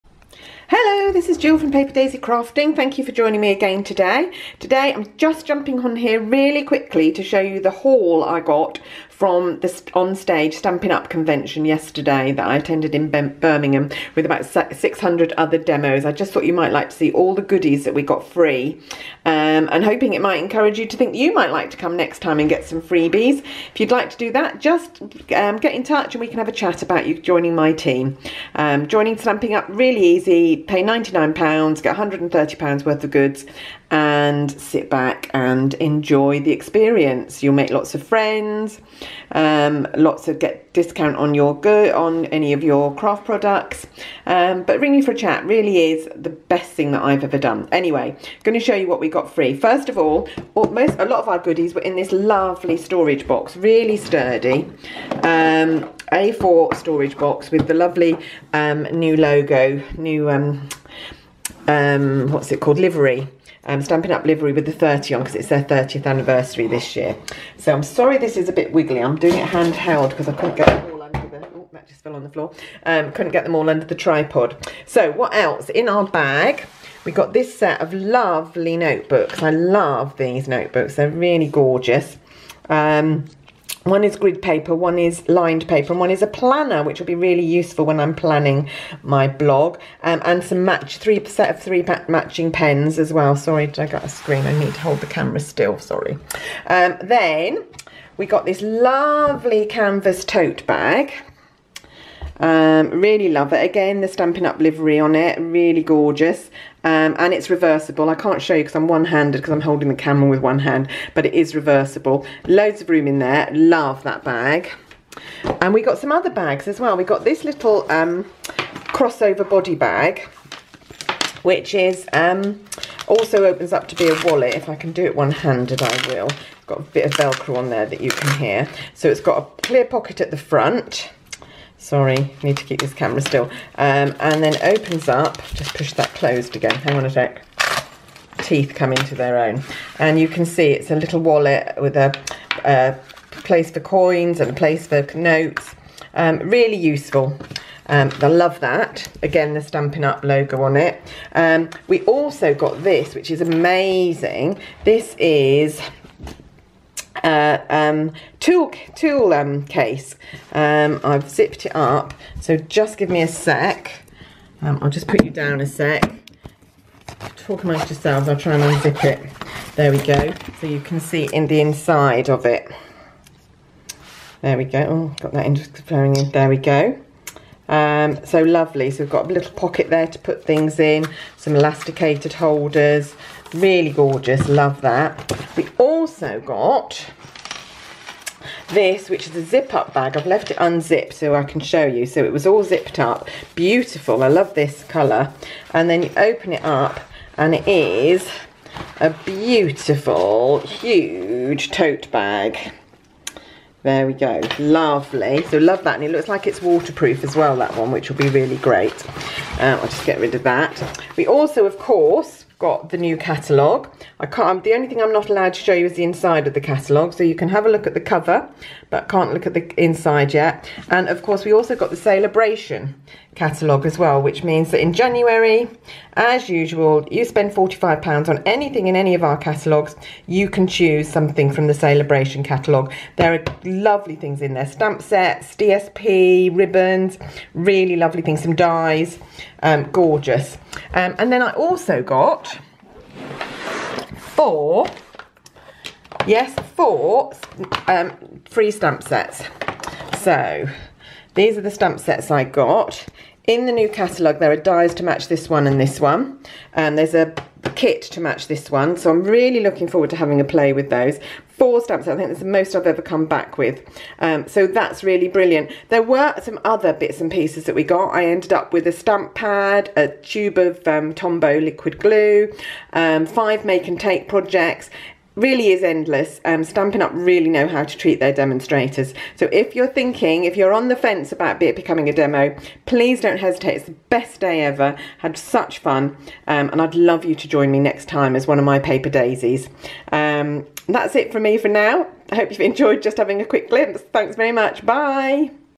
Back from On Stage with a whole host of freebies - come see!
Please excuse the “wobbling” and out of camera shots in this video – I had to film one handed!
Independent Stampin' Up! UK Demonstrator Jill Chapman shares a project with you. Come and see how easy it is to make this lovely card using the products from Stampin' Up!

To JOIN my team of Paper Daisy Stampers – I’d be so happy to welcome you to the team 
I hope you enjoyed this project - I will be back with another one soon :-)

Jill xx